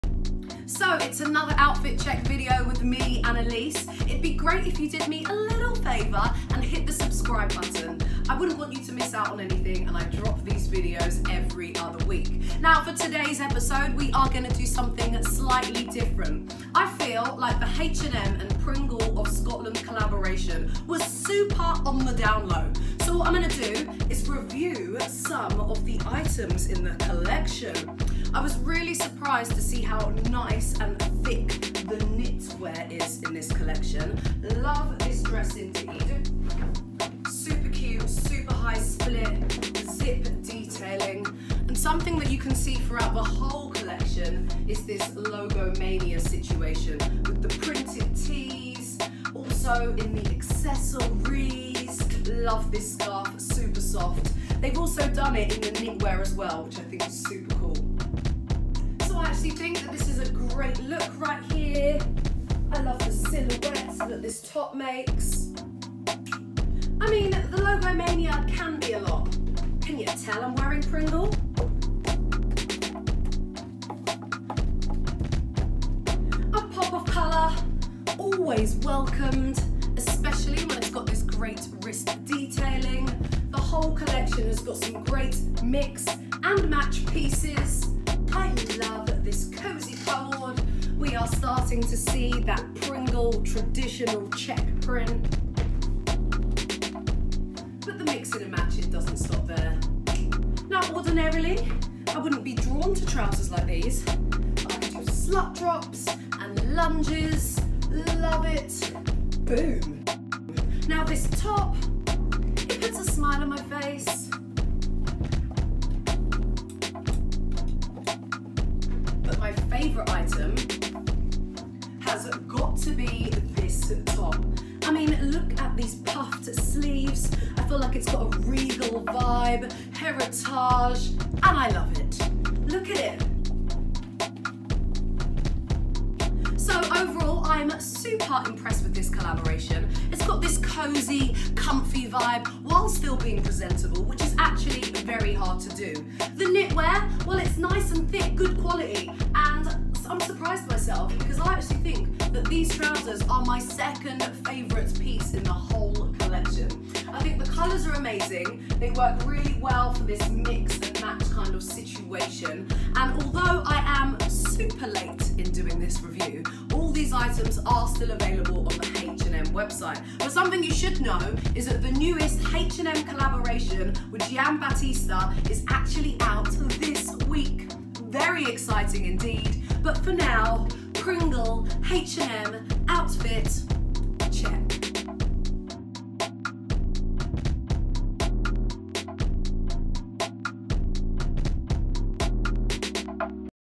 H&M x Pringle... What do you think? Outfit Check
So the launch of H&M’s collab with Pringle of Scotland was totally missed by me! I stumbled across the collection well after the end of September launch but I was super impressed! I had to do an Outfit Check review! It’s a cozy, comfy, casual collection that’s great for those winter mornings but definitely not suitable for the club! Yes it’s true I’m squeezing this video into the Outfit Check schedule because the newest H&M collab with Giambattista Valli drops later this week. However you'll be pleased to hear that although slightly limited all items featured in this video are still available (of were when I filmed!)

All looks from H&M.